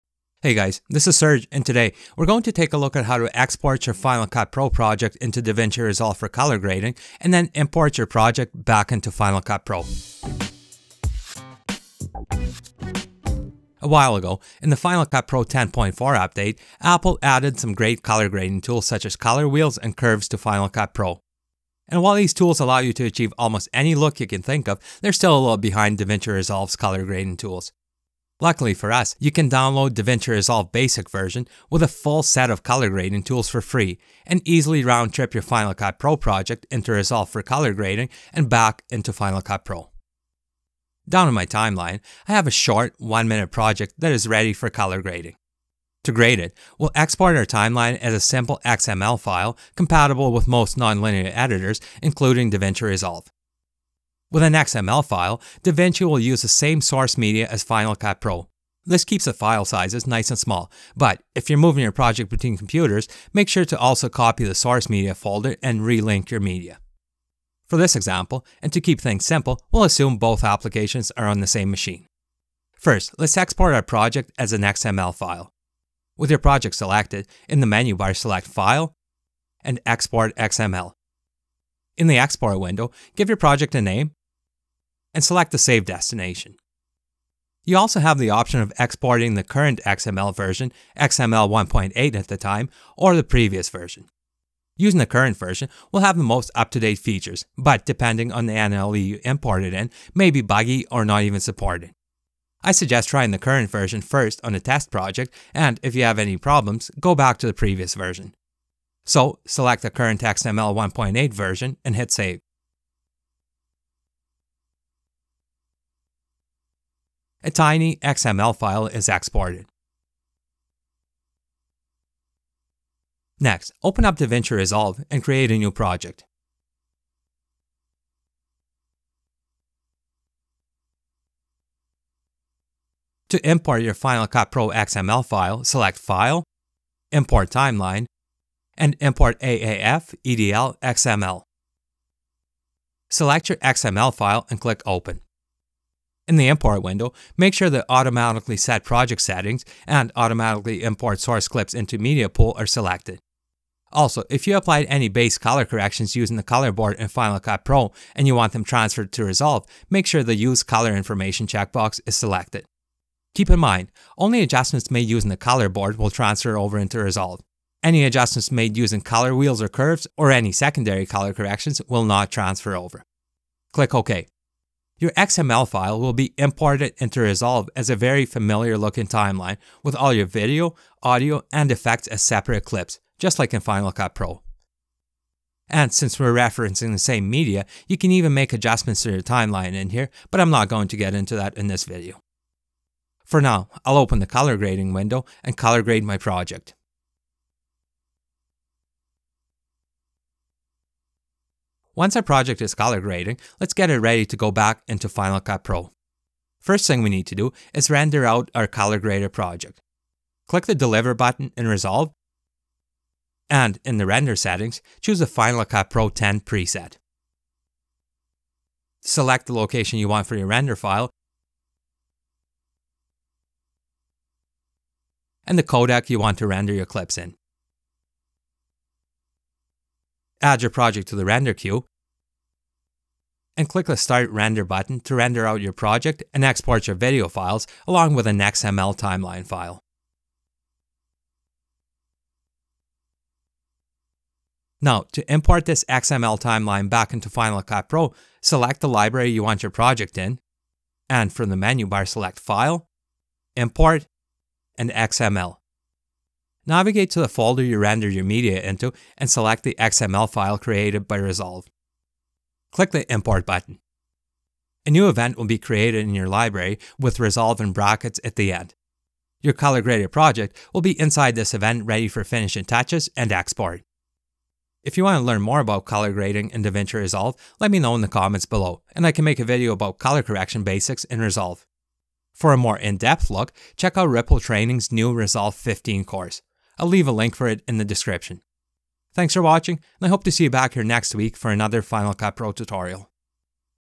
FCPX Project to DaVinci Resolve and Back
In this video, we'll take a look at how to export your Final Cut Pro timeline into DaVinci Resolve for color grading, and back into Final Cut Pro for finishing touches.

Final Cut Pro has some great color grading tools, but still not as good as DaVinci Resolve. If you prefer to use DaVinci Resolve for color grading, there is an easy way to export a Final Cut Pro timeline as an XML file, import it into Resolve for color grading, and round trip it back into Final Cut Pro for finishing touches and export.

SERVICES I USE TO MAKE VIDEOS